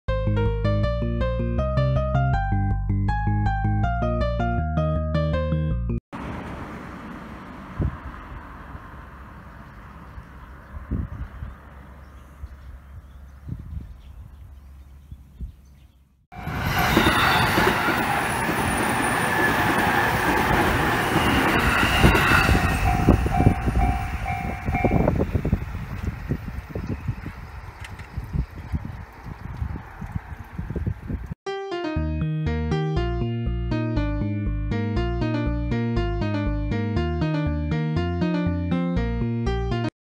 JR総武本線の踏切（成東ー松尾）千葉県
JR総武本線の成東駅と松尾駅の間の踏切です。